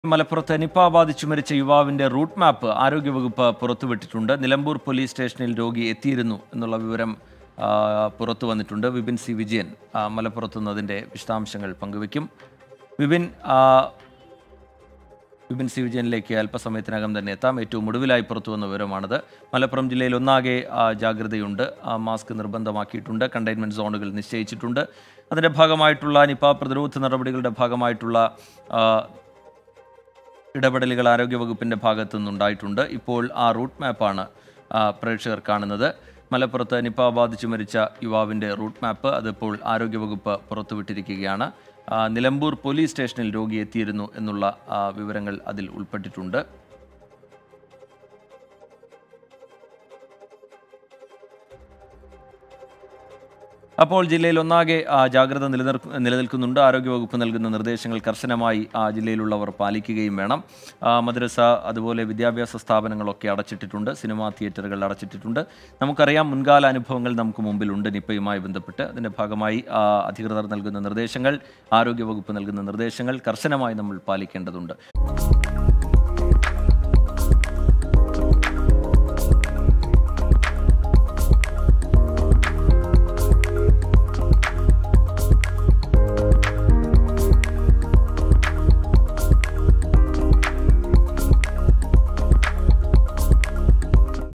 മലപ്പുറത്ത് നിപ ബാധിച്ച് മരിച്ച യുവാവിന്റെ റൂട്ട് മാപ്പ് ആരോഗ്യ വകുപ്പ് പുറത്തുവിട്ടു
മലപ്പുറത്ത് നിപ ബാധിച്ച് മരിച്ച യുവാവിന്റെ റൂട്ട് മാപ്പ് ആരോഗ്യ വകുപ്പ് പുറത്തുവിട്ടു. നിലമ്പൂർ പൊലീസ് സ്റ്റേഷനിൽ രോഗിയെ എത്തിയിരുന്നു.
Malappuram|  Nilambur police station | Nipah